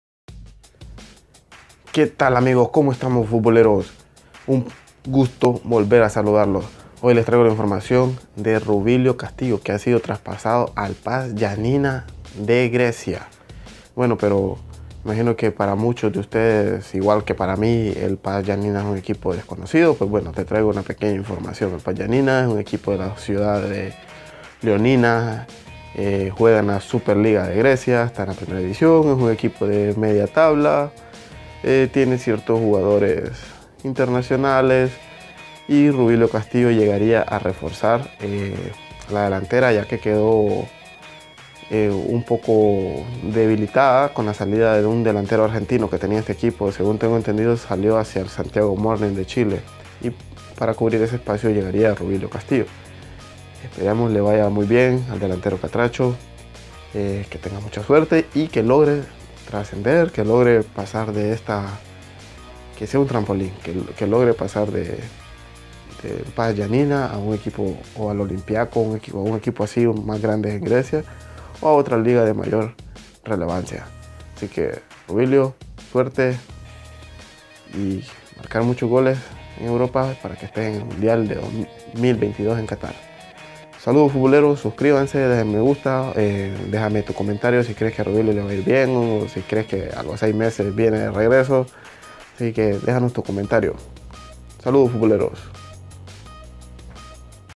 RUBILIO CASTILLO al PAS GIANNINA de Grecia
El Delantero Hondureño Rubilio Castillo es el nuevo fichaje del Pas Giannina de la Super Liga Griega.